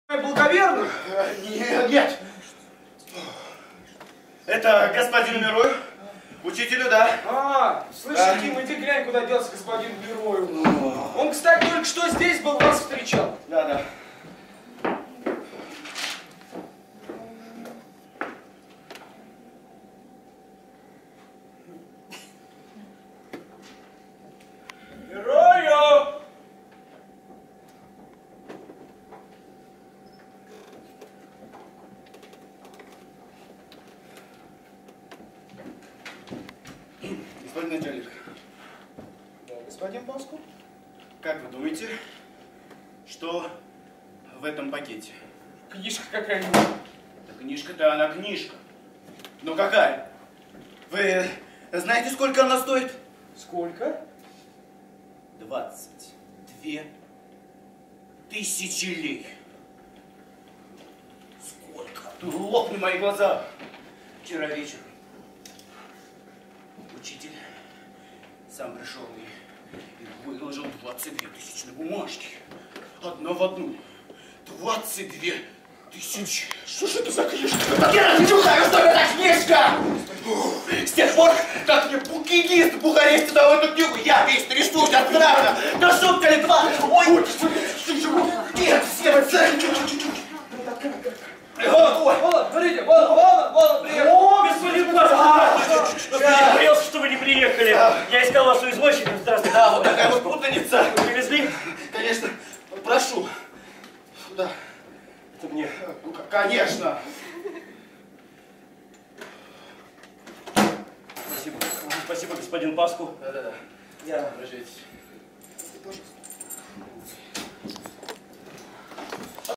«...книга. Но какая? Двадцать две тысячи лей. Лопни моя глаза!» (Безымянная звезда)
Спектакль по пьесе М.Себастиана  «Безымянная звезда»
(фрагменты спектакля, запись от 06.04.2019)
Паску - Саро Балаян
Начальник вокзала - Фёдор Мишеев
Учитель - Станислав Сошников
Иким - Сергей Смолов
Режиссер-постановщик: А.В.Сергеев, С.Г.Сошников
Мастерская народного артиста РФ Виталия Коняева
Техническая съемка на камеру Sony DSC-RX100

Паску (таинственно). Господин начальник! Как, по-вашему, что в этом пакете?
Начальник. Что там может быть? Книга, наверное.
Паску. Так и есть—книга. Но какая? (Таинственно.) Знаете, сколько она стоит? Двадцать две тысячи лей.
Начальник. Сколько?
Паску. Двадцать две тысячи.
Начальник. Да ну? Не может быть.
Паску. Лопни моя глаза. (Быстро объясняя.) Учитель приходил ко мне вчера вечером и вручил мне двадцать две тысчонки по тысяче… ровно двадцать две штуки.
Начальник (озабоченно). Какая же это может быть книга?
Паску. А мне откуда знать? Мне ее дал букинист в Бухаресте, я ее и привез. Дрожал за нее всю дорогу. Это же не шутка — двадцать две тысячи!
Начальник. Двадцать две тысячи!